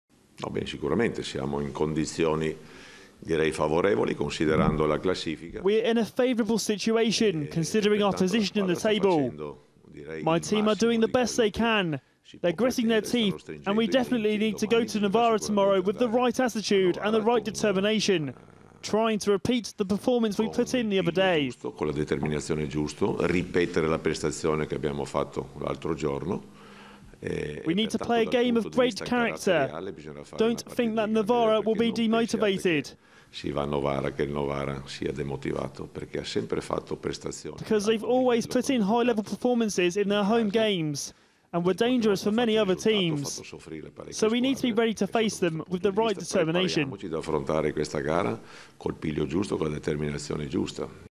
Lazio coach Reja focused ahead of trip to doomed Novara
Third-placed Lazio are not expecting an easy ride when they visit doomed Novara on Wednesday. Coach Edoardo Reja believes his Lazio team are doing the best they can as they look to secure the last Champions League qualifying spot in Serie A. The fight for top spot is between Juventus and Milan, while the Romans are just three points clear of Udinese in fourth. Novara are eleven points adrift of safety with just five games to go, and must surely be turning their thoughts to next season in Serie B.